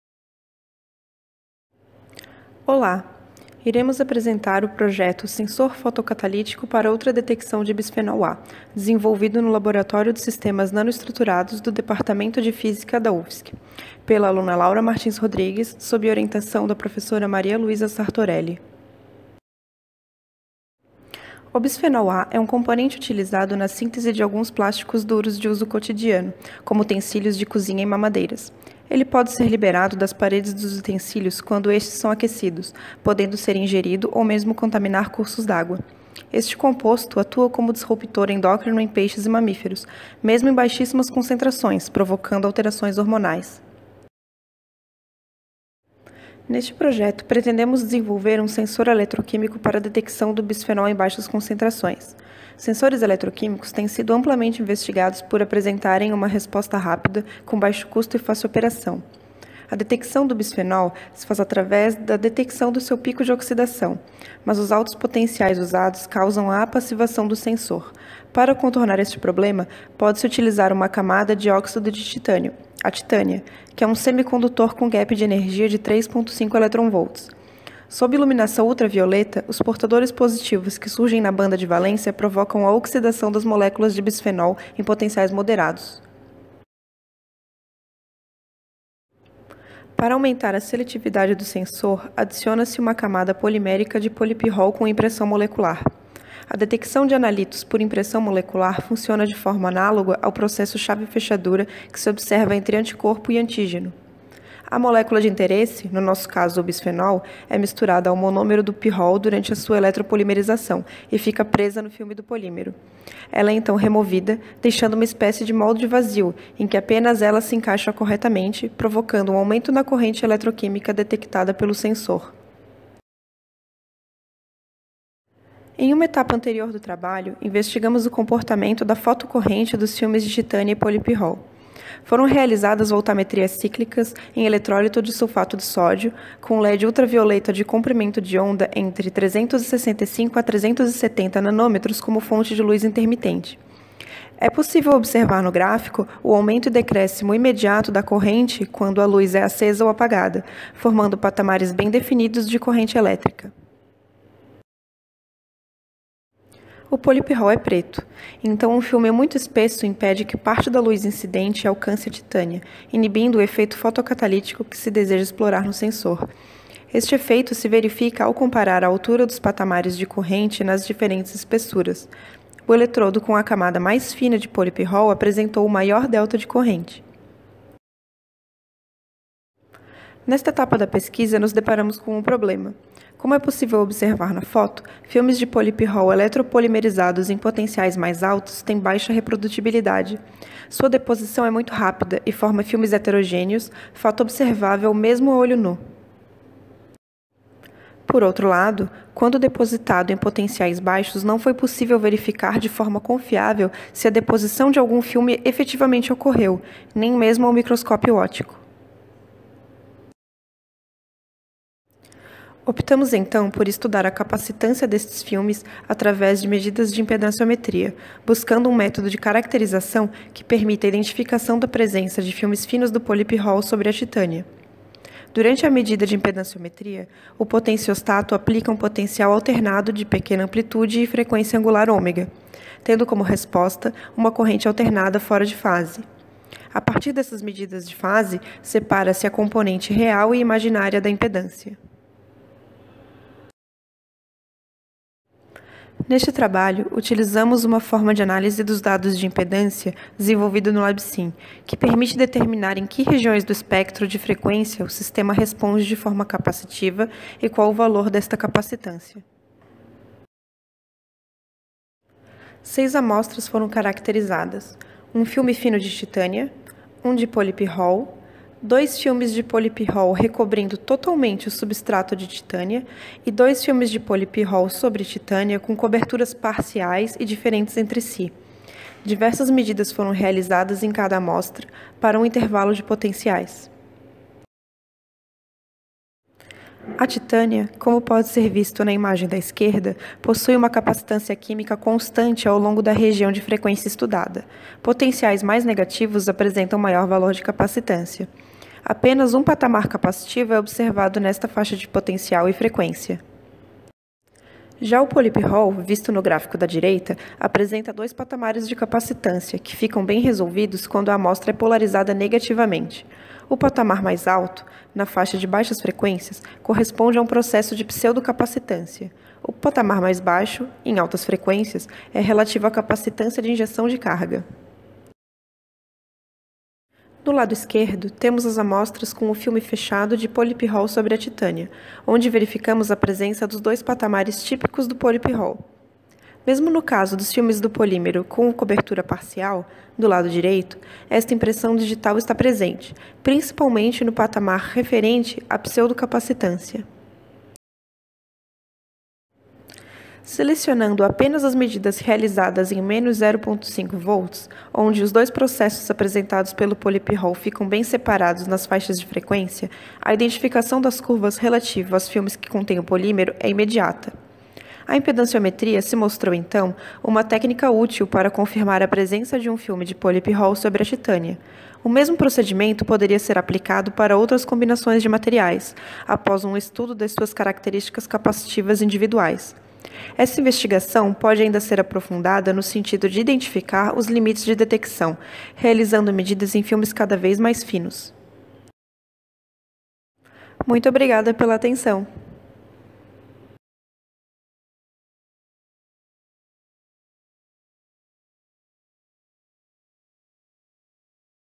LAURA MARTINS RODRIGUES (27º SIC UFSC)
Sensor fotocatalítico para ultradetecção de bisfenol A

Bolsista: Laura Martins Rodrigues
Orientadora: Maria Luisa Sartorelli

Centro de Ciências Físicas e Matemáticas
Departamento de Física
Laboratório de Sistemas Nanoestruturados (LabSin)

Área: Ciências Exatas e da Terra
Sub-área: Física da Matéria Condensada

Universidade Federal de Santa Catarina

Resumo: O objetivo principal deste trabalho, realizado no Laboratório de Sistemas Nanoestruturados (LabSin) é desenvolver um sensor fotoeletrocatalítico de bisfenol-A (BPA), a partir da impressão molecular de polipirrol (PPY). O PPY é sintetizado sobre titânia, o que permite a detecção fotocatalítica do BPA sem causar sua polimerização sobre o sensor, efeito indesejado que ocorre em decorrência dos altos valores de potencial aplicados na detecção. Na etapa anterior do trabalho investigamos o comportamento da fotocorrente dos filmes de titânia e polipirrol, e verificou-se que eletrodos com camadas mais finas de polímero apresentam maior delta de corrente. Não é possível, no entanto, verificar oticamente a efetiva deposição de filmes ultra-finos de polipirrol sobre a titânia. Se fez necessário, nesta etapa do trabalho, investigar uma forma de caracterização. Optou-se por estudar a capacitância destes filmes através de medidas de impedanciometria, utilizando uma forma de análise dos dados de impedância desenvolvida no LabSiN, que permite determinar em que regiões do espectro de frequência o sistema responde de forma capacitiva, e qual o valor dessa capacitância.